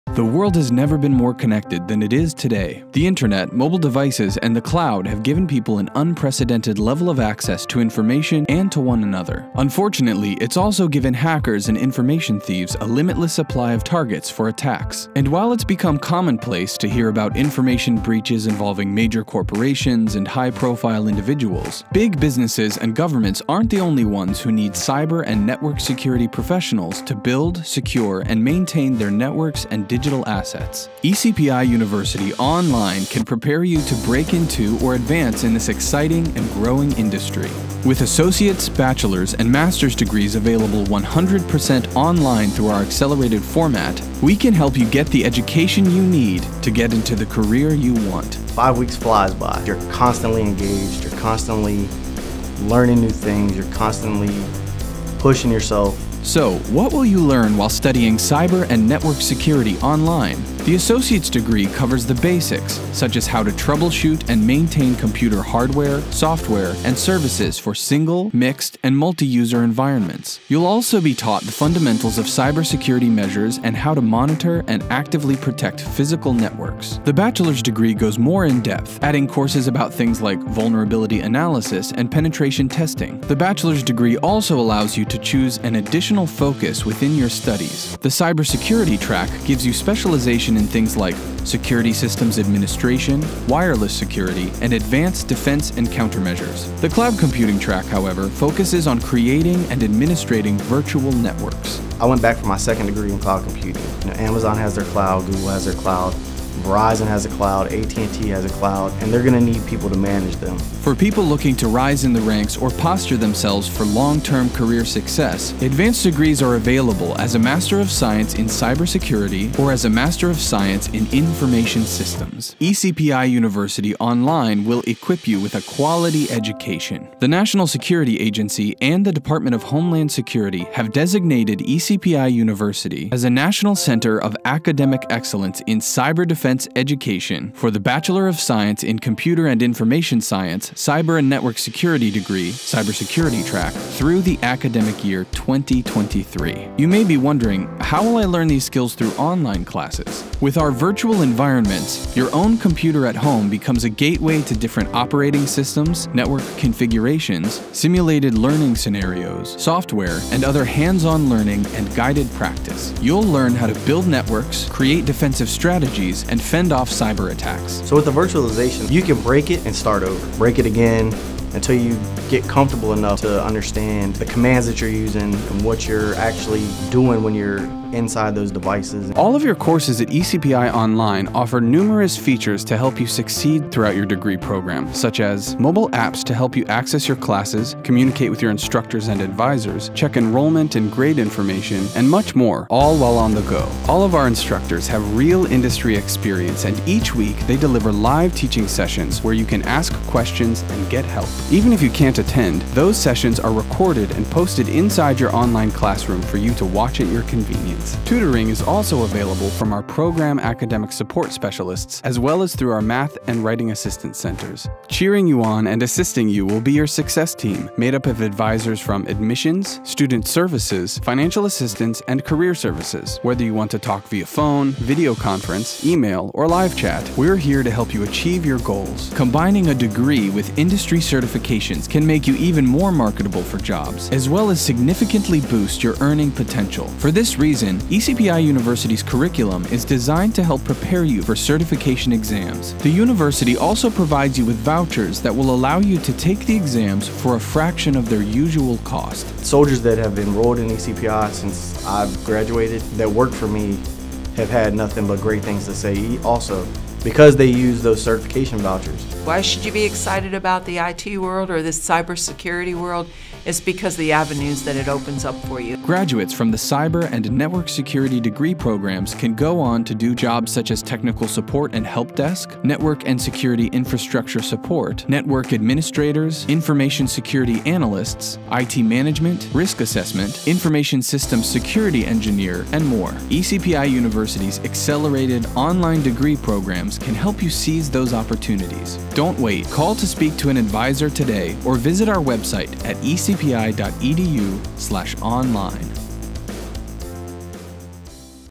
Cyber and Network Security Overview at ECPI University Online
Do you want to stop cyber-crime before it starts? If preventing, detecting, and battling digital crime is important to you, a career in cyber security may be the perfect fit, and a Computer and Information Science degree with a major in Cyber and Network Security could be a great way to start! As a cybersecurity specialist, you could be on the front lines of cyber warfare and defense, protecting vital computer systems against inside and outside threats alike, including social engineering, hackers, malware, spyware, and viruses.

For information on program success, costs, and other important facts,

You could earn a Bachelor of Science Degree in Computer and Information Science with a Cyber and Network Security major in just 2.5 years through ECPI University’s year-round degree program.

For more information about ECPI University or any of our programs